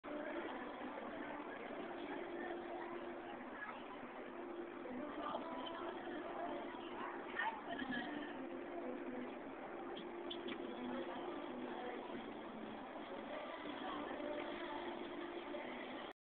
Waveswinger at Kings Dominion
Cameraphone video of the Waveswinger swing ride at Kings Dominion.  Taken on opening day, 2008